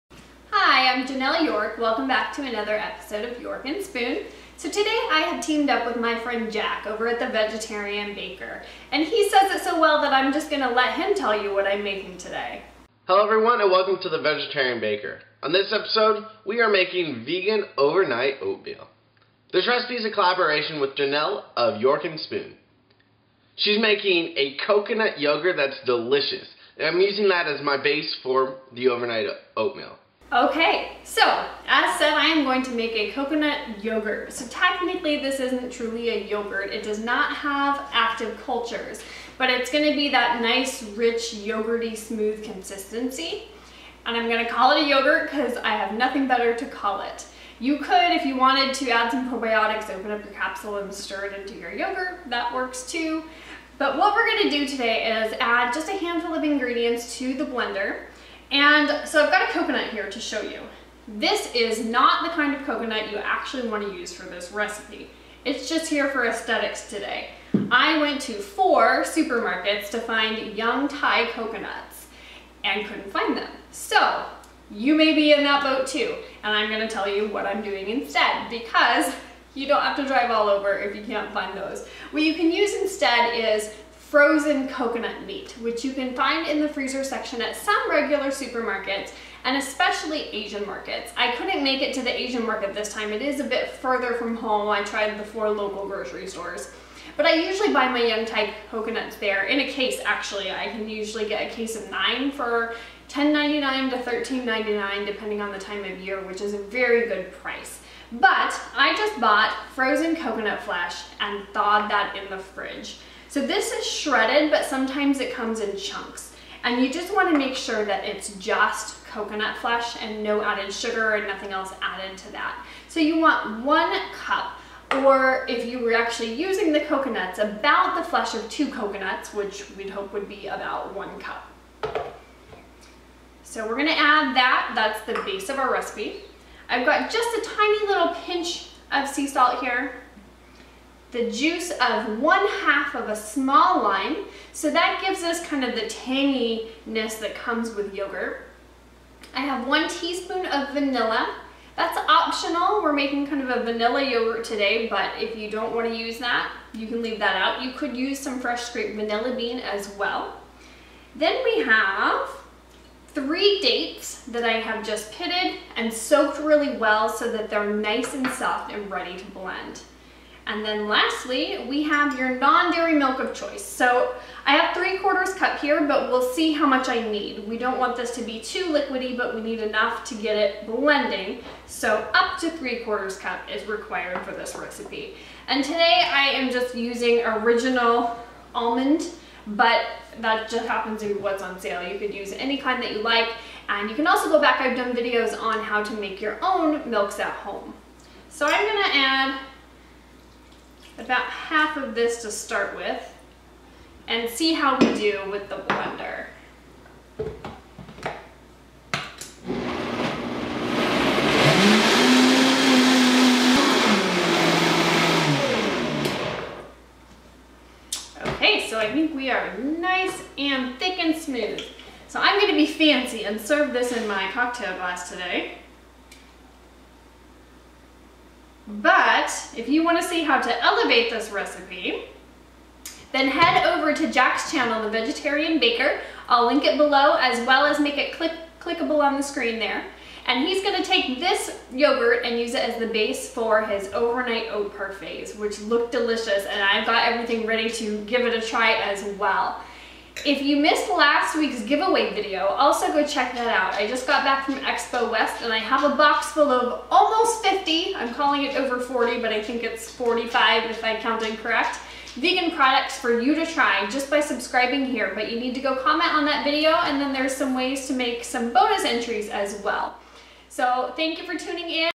Fresh Coconut Yogurt / Overnight Oats Collab w/ The Vegetarian Baker
This cheater "yogurt" gives the rich, creamy consistency of a decadent yogurt. See how to make it in this episode of York & Spoon with Jenelle York. Then head to The Vegetarian Baker's channel where he uses it to make Overnight Oats